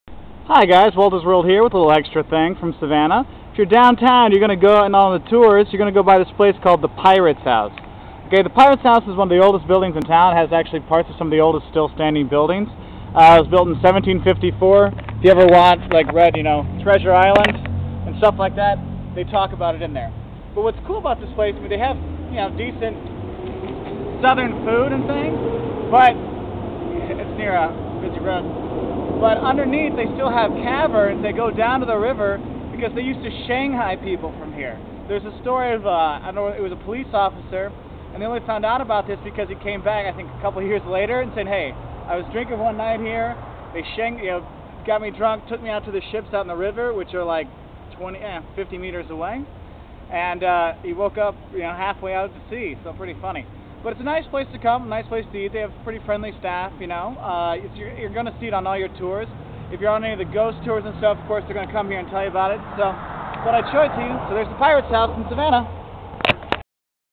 Savannah: The Pirates House Where Black Beard Died
The Pirates House is where (supposedly) Robert Louis Stevenson wrote part of Treasure Island. Neat place to eat. See the other Pirates House video for an inside look.